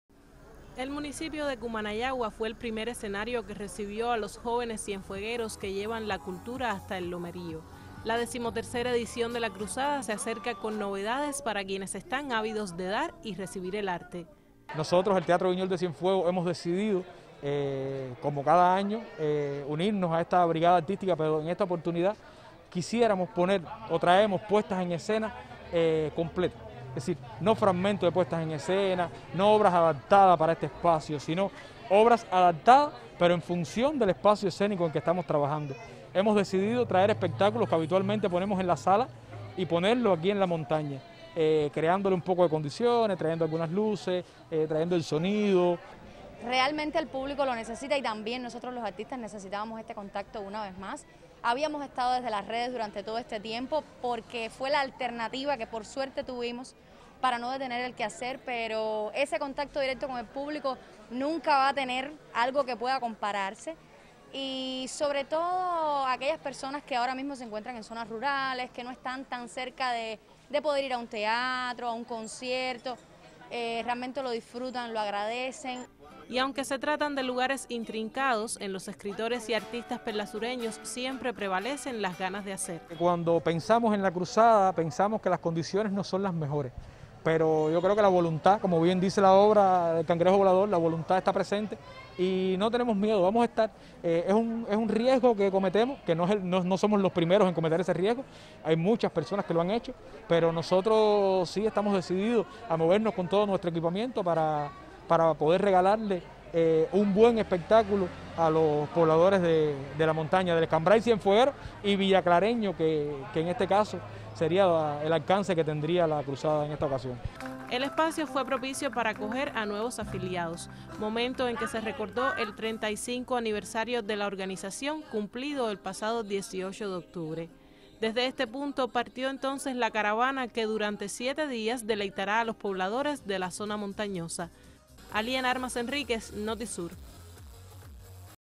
Inició cruzada artístico literaria en Cienfuegos
La cruzada artístico literaria recorrerá una treintena de asentamientos del Escambray y el litoral sur de la provincia. El homenaje a los Hermanos Luis y Sergio Saíz es siempre una de las motivaciones para este certamen, que una vez más busca una mayor cercanía con los pobladores de zonas montañosas.